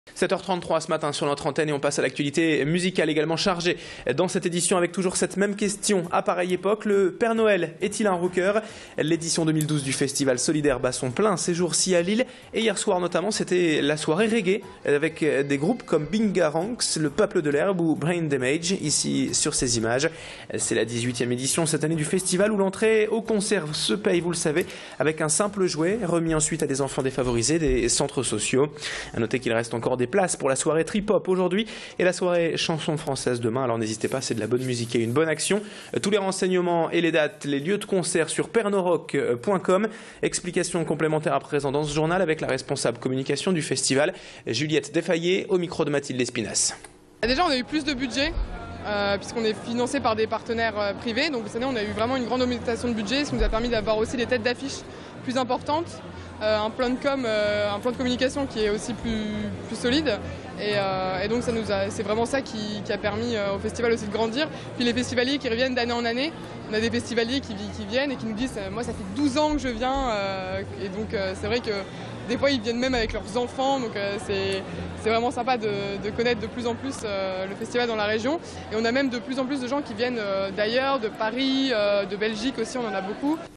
Le Père Noël est-il un Rocker 2012 : Le festival bat son plein (Lille)
Le festival caritatif Le Père Noël est-il un Rocker 2012 bat son plein à Lille. Retour sur les raisons du succès  de l'édition 2012

Sites webs/blogs, copiez cette vidéo avec le bouton "Intégrer" !